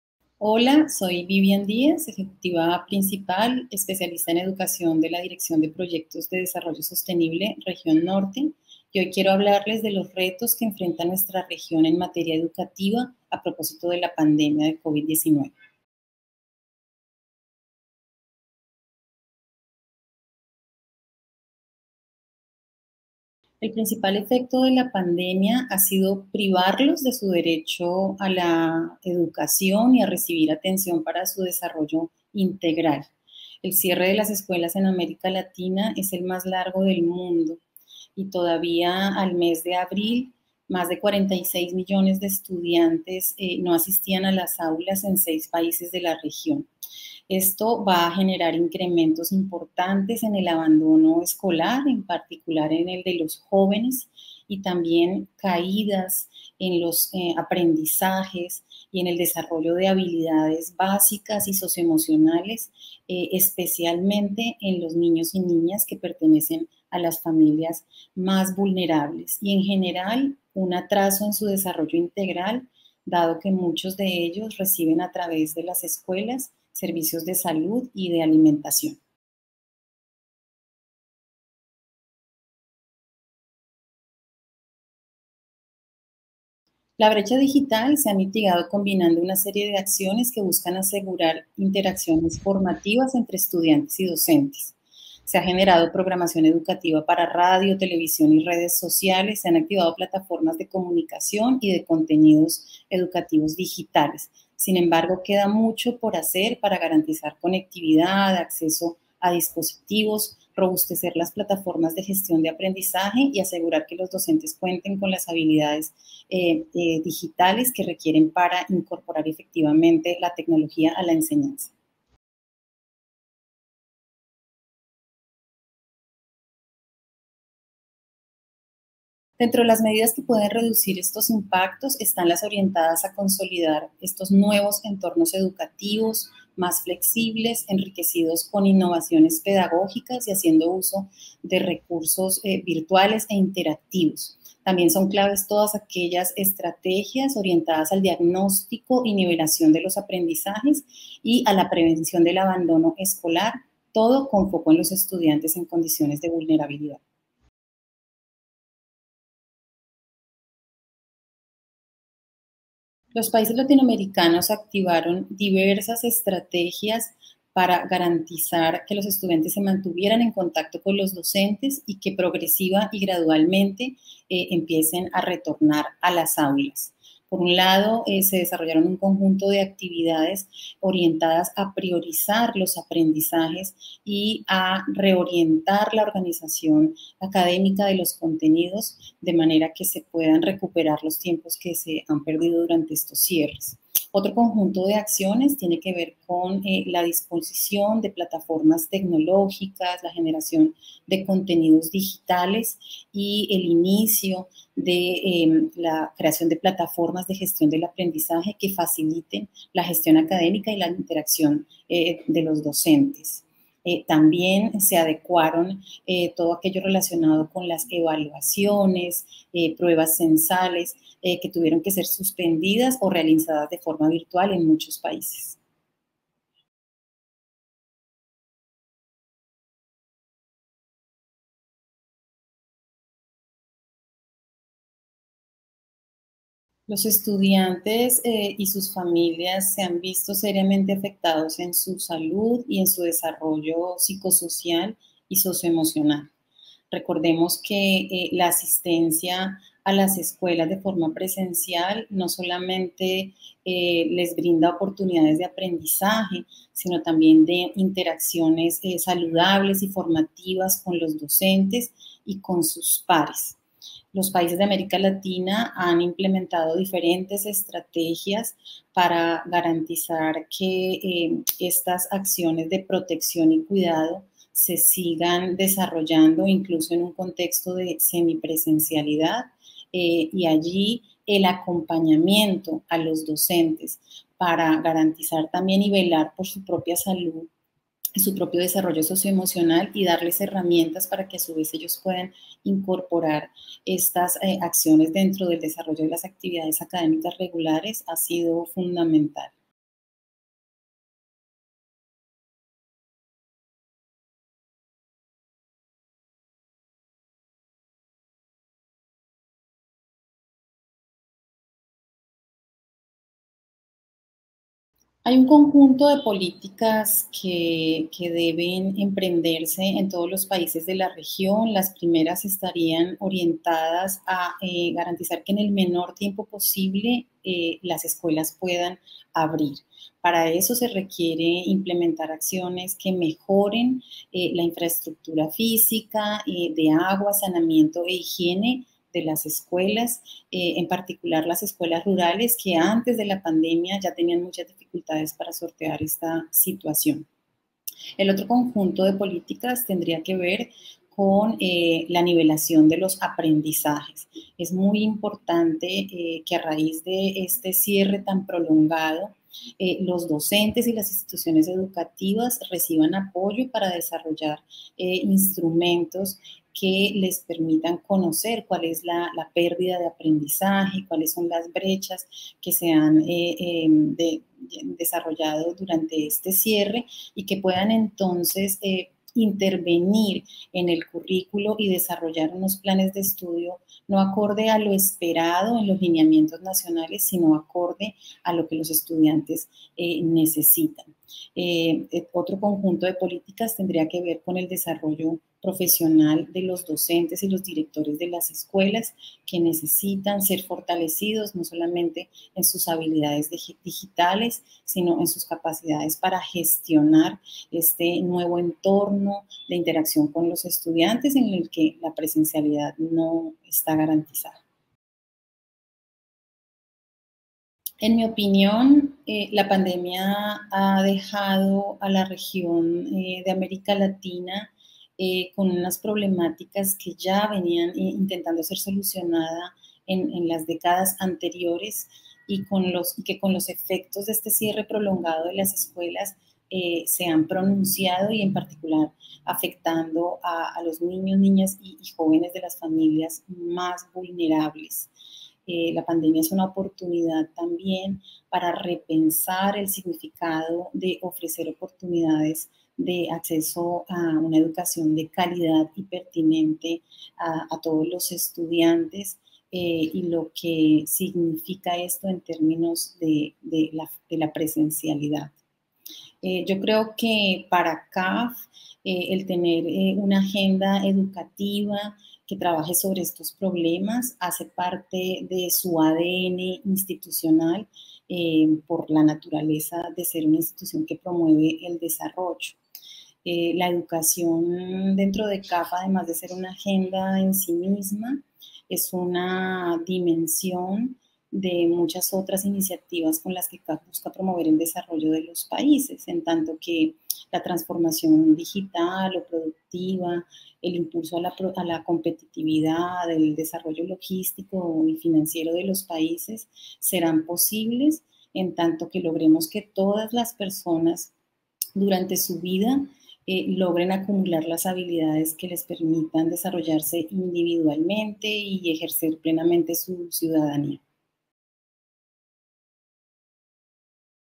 Educación en pandemia: ¿Año perdido para América Latina? Bibiam Díaz - CAF (entrevista completa)
Bibiam Díaz, Ejecutiva Principal, Especialista en Educación de la Dirección de Proyectos de Desarrollo Sostenible, Región Norte de CAF - banco de desarrollo de América Latina , nos comenta cuáles son los principales efectos de la pandemia del Covid-19 en la educación de niños, niñas y jóvenes de América Latina, cómo respondieron los países latinoamericanos a esta emergencia educativa, y qué tipo de medidas pueden reducir los impactos negativos del cierre de las escuelas en la región.